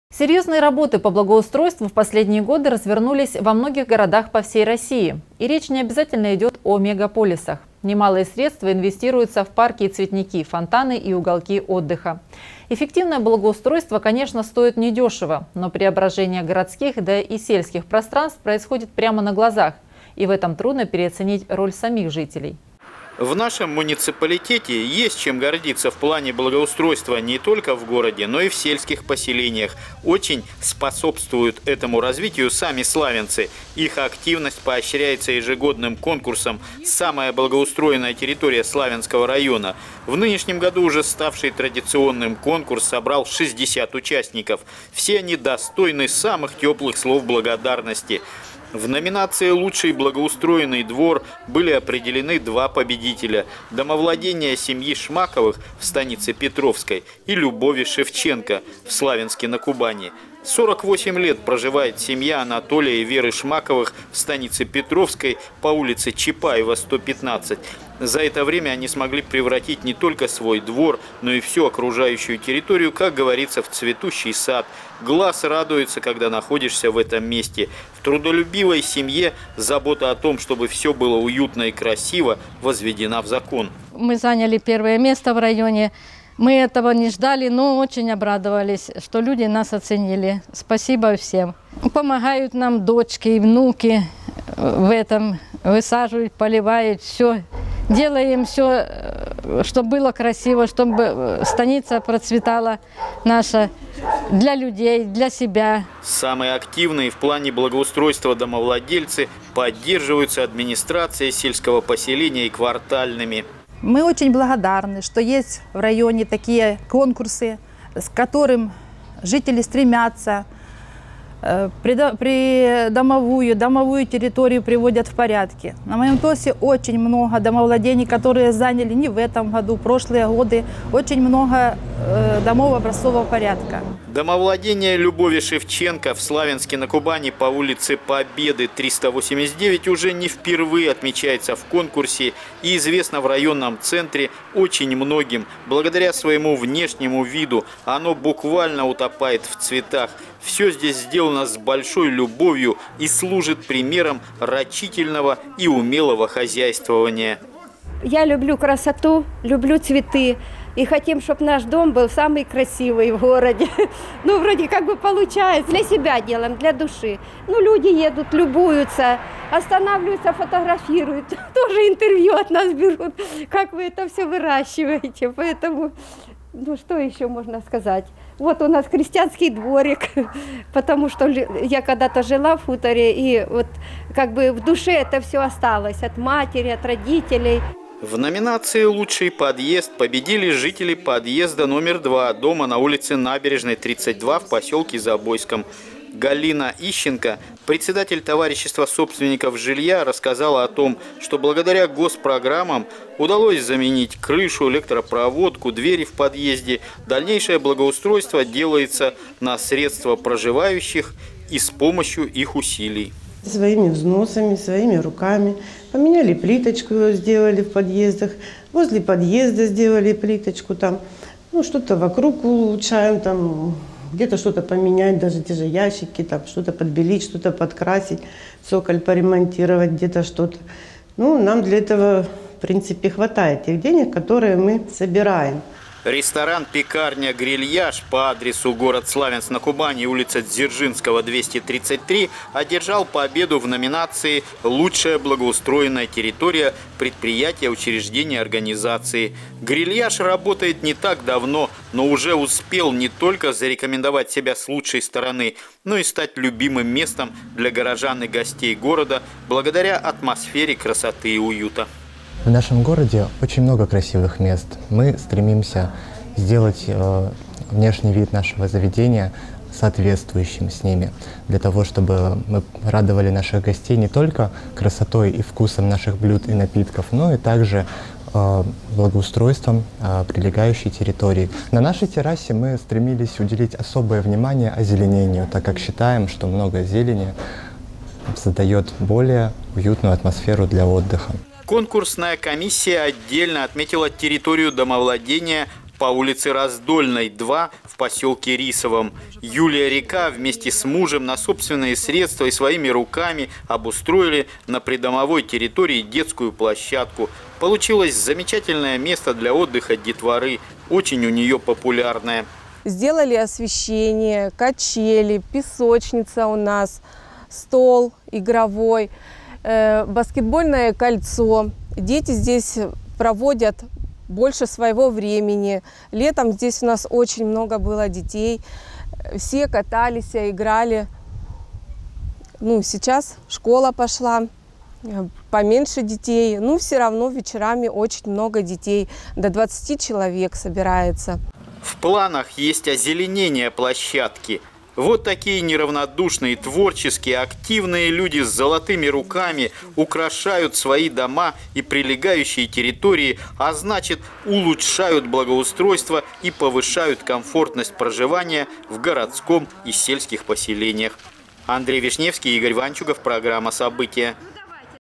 Самая благоустроенная территория Славянского района
Серьезные работы по благоустройству в последние годы развернулись во многих городах по всей России – и речь не обязательно идет о мегаполисах. Немалые средства инвестируются в парки и  цветники, фонтаны и уголки отдыха. Эффективное благоустройство, конечно, стоит недешево, но преображение городских, да и сельских  пространств происходит прямо на глазах. И в этом трудно переоценить роль самих жителей